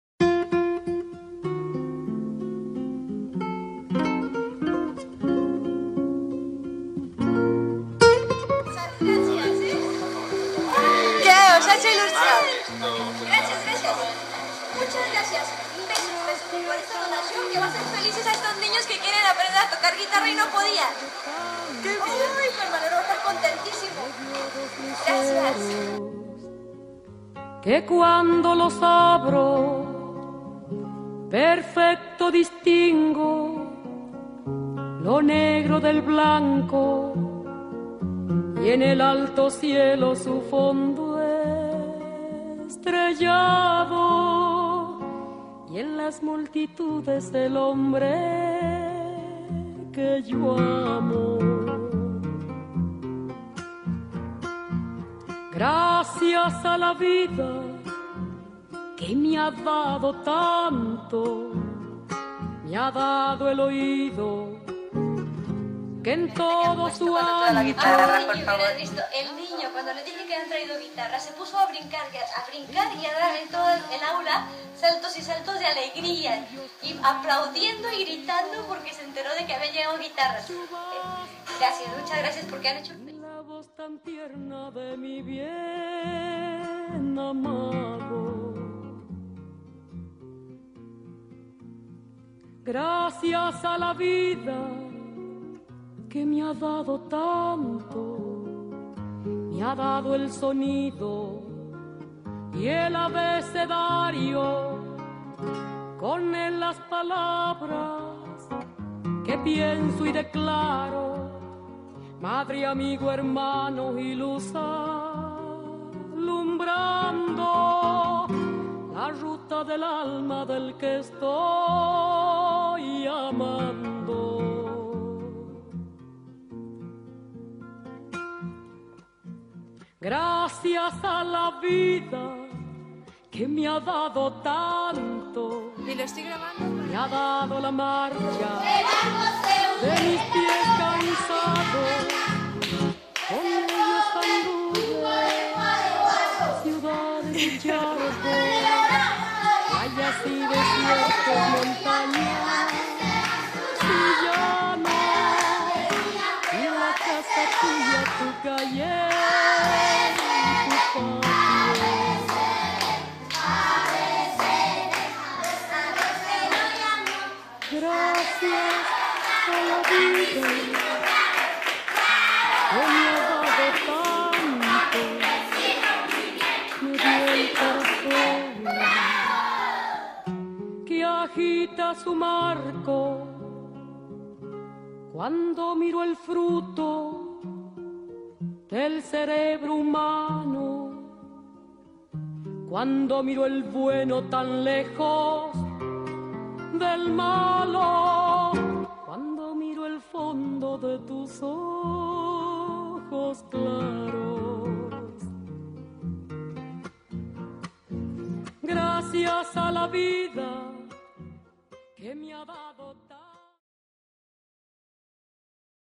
Malabo Junio 2012
He realizado este vídeo en agradecimiento a todos los que contribuís a que estas misioneras puedan continuar su maravillosa labor en Basakato Malabo

Gracias a mi entregada tripulación que me ayudó a llevar todo lo que aportásteis.

Gracias